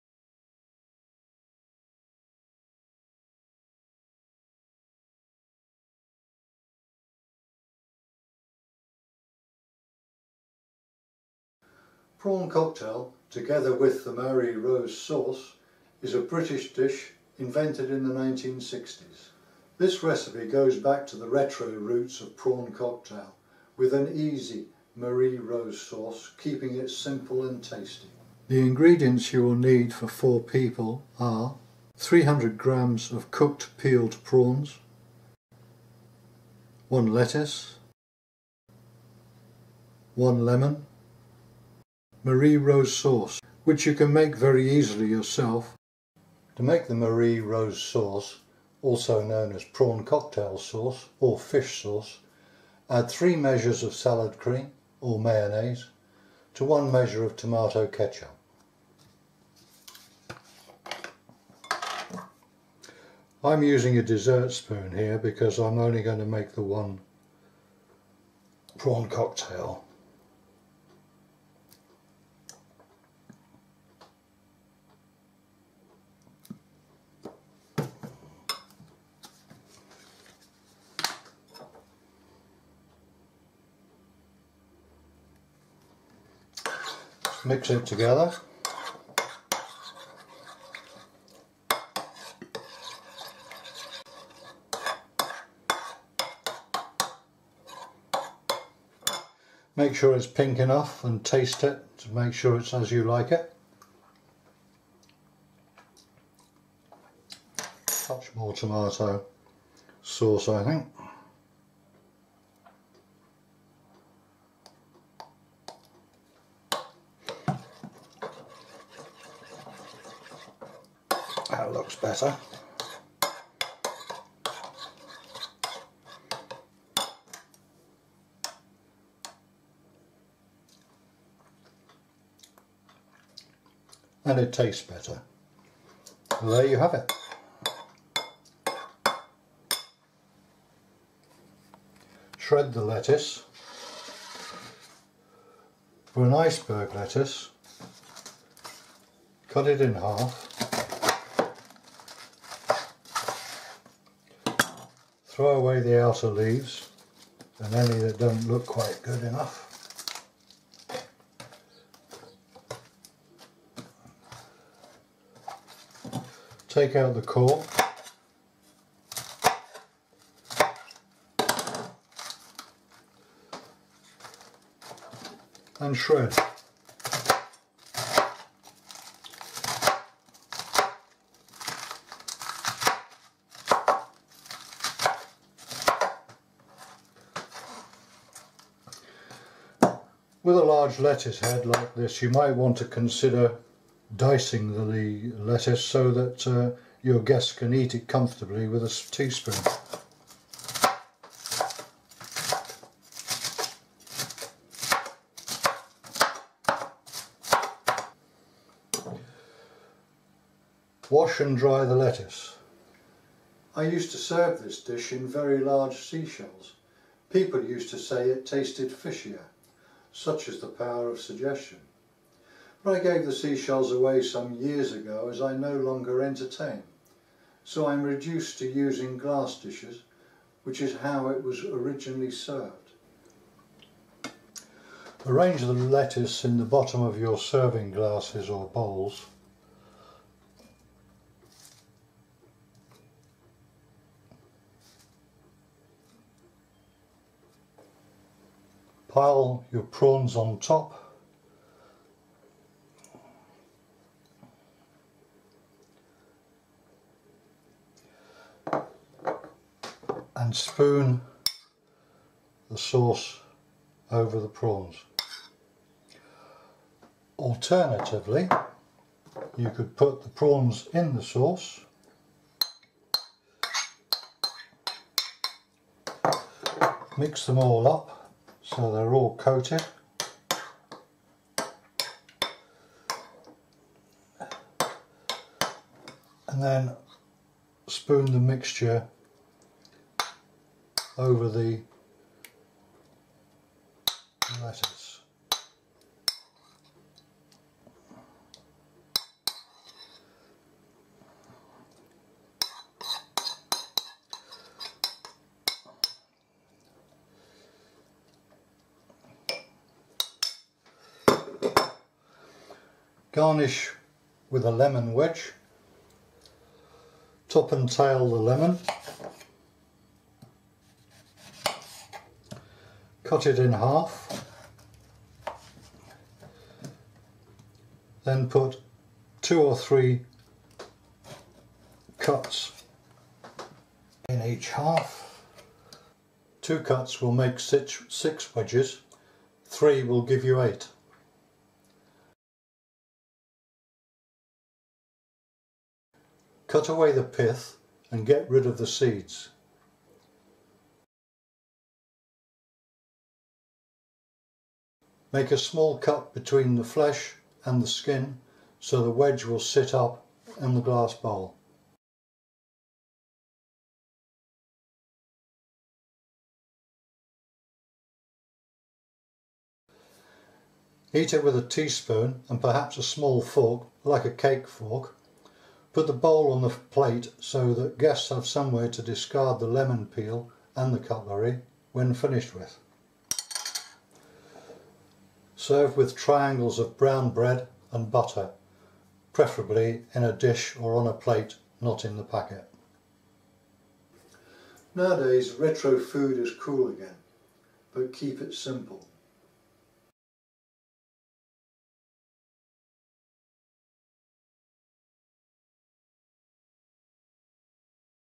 I'm not a cook - 5 - Easy Prawn Cocktail
Prawn cocktail is a seafood dish made with cooked prawns, lettuce and Marie Rose sauce, served with triangles of brown bread and butter.
Prawn cocktail together with the Marie Rose sauce, is a British dish invented in the 1960’s and was widely used as the first course of many dinner parties in the 1970’s and 80’s.

INGREDIENTS
Cooked peeled prawns, (300 grams / 9 oz. will serve 4 people). If you are using frozen prawns remember to thaw them slowly. Preferably, put them in a fridge the night before you are going to use them.
Lettuce
Salad cream or mayonnaise
Tomato ketchup

METHOD
Make the Marie Rose sauce.
For each person mix together in a bowl
3 tablespoons salad cream or mayonnaise
1 tablespoon tomato ketchup.
Check for colour and taste and adjust as necessary. If using mayonnaise add lemon juice for sharpness.
Shred the lettuce
Arrange the lettuce in the bottom of your serving glasses or bowls
Pile your peeled prawns on top, then spoon over the Marie Rose sauce. Or you can mix the prawns into the sauce to coat each one then spoon the mixture on top of the lettuce.
Add  a sprinkling of paprika (optional) to make it look interesting
Serve with a wedge of lemon so people can make the sauce sharper if they want to and triangles of brown bread and butter.
Eat with a teaspoon and small fork.